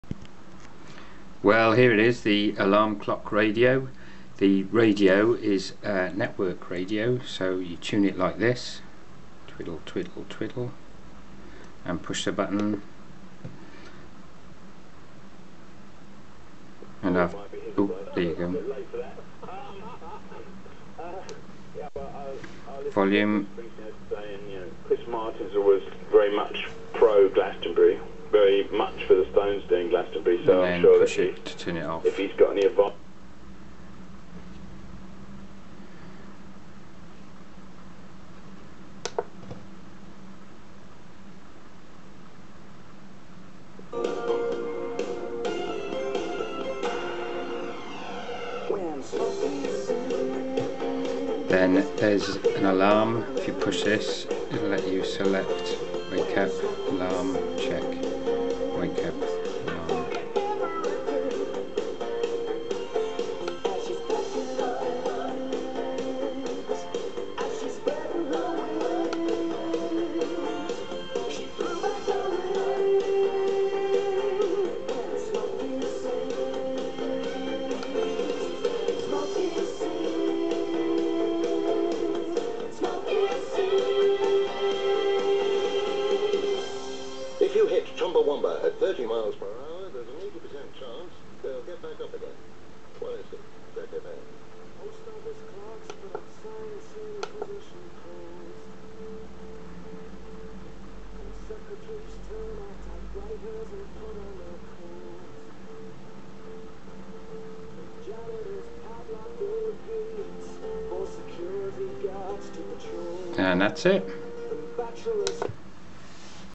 Raspberry Pi - Network Alarm Clock Radio - NACRintro
NACR - Network Alarm Clock Radio
Features
Uses GPIO pins to read two rotary encoder knobs.
Uses I2C bus to drive four Adafruit 8x8 led matrix displays.
Plays network radio streams (via VLC)